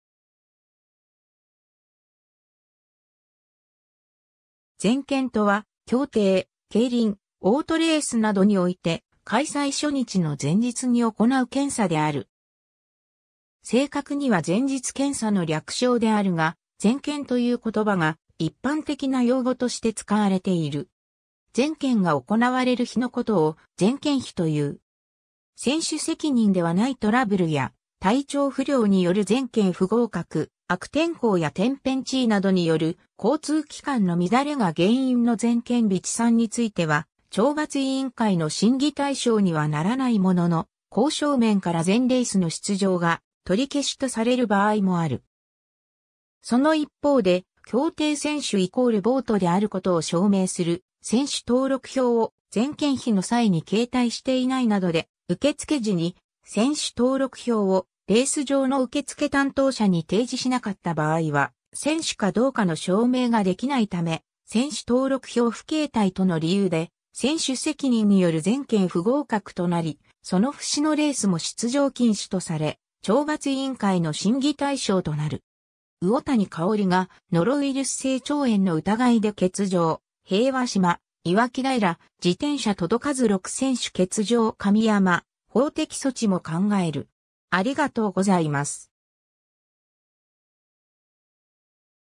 前検
前検（ぜんけん）とは、競艇、競輪、オートレースなどにおいて、開催初日の前日に行う検査である。
 正確には前日検査の略称であるが、前検という言葉が一般的な用語として使われている。
前検が行われる日の事を前検日と言う。
 選手責任ではないトラブルや体調不良による前検不合格、悪天候や天変地異などによる交通機関の乱れが原因の前検日遅参については、懲罰委員会の審議対象にはならないものの、公正面から全レースの出場が取り消しとされる場合もある（ただし交通機関の乱れなどによる選手責任外での前検日遅参の場合は、止むを得ない事情と判断され、翌日からのレースに出場が可能となる場合もある）。
その一方で、競艇選手=ボートレーサーであることを証明する選手登録票（選手登録票=一般自動車の運転免許証に相当する物）を前検日の際に携帯していないなどで、受付時に選手登録票をレース場の受付担当者に呈示しなかった場合は、選手かどうかの証明ができないため、選手登録票不携帯との理由で選手責任による前検不合格となり、その節のレースも出場禁止とされ、懲罰委員会の審議対象となる。
 ^ 魚谷香織がノロウイルス性腸炎の疑いで欠場／平和島 ^ 【いわき平】自転車届かず６選手欠場　神山「法的措置も考える」